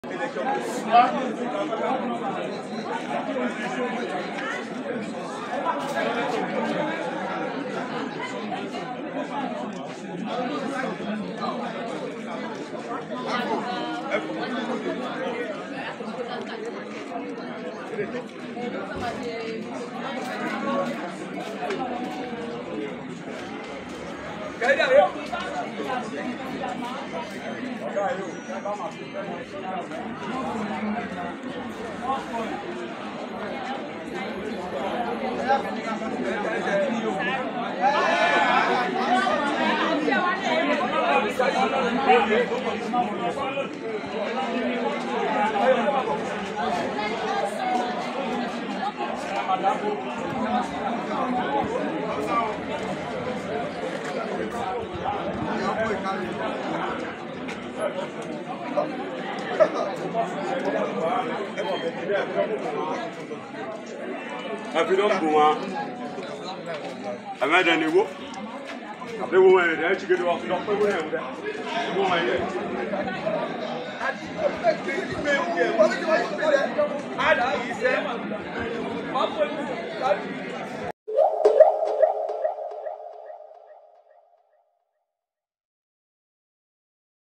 Vote Buying In Ondo Election With Impunity
Party Agents Are Buying Votes Openly And Negotiating With Electorates In Owo, Ondo Statem